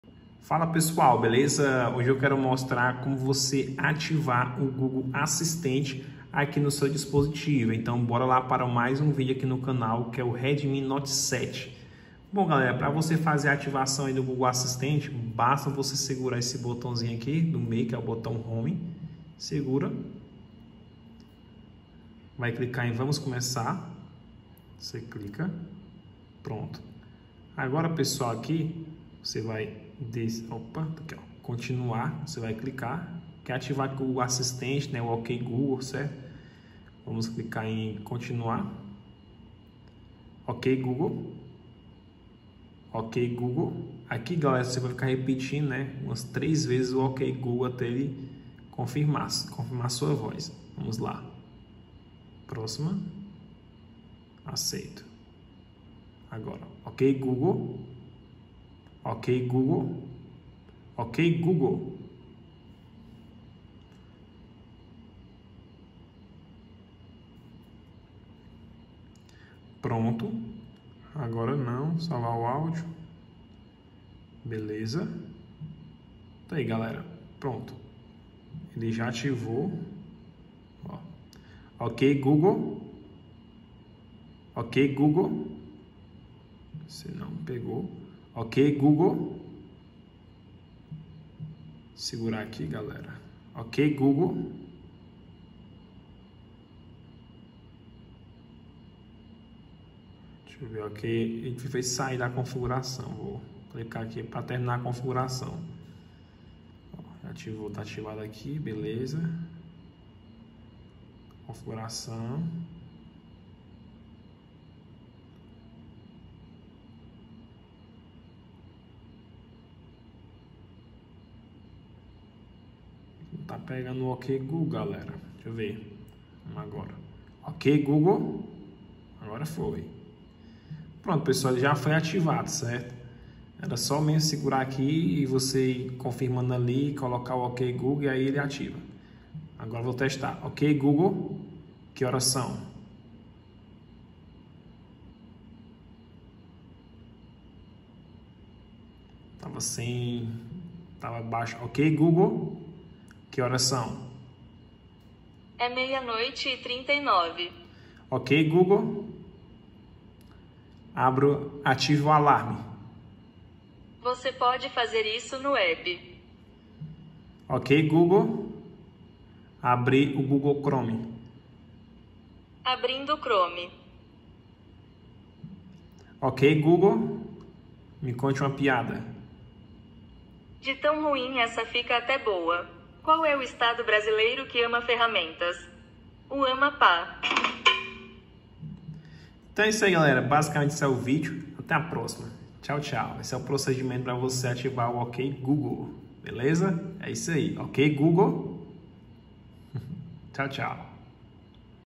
Como ativar o Google Assistente no Redmi Note 7
Hoje em mais um vídeo aqui no canal eu quero mostrar como ativar o Google Assistente no Redmi Note 7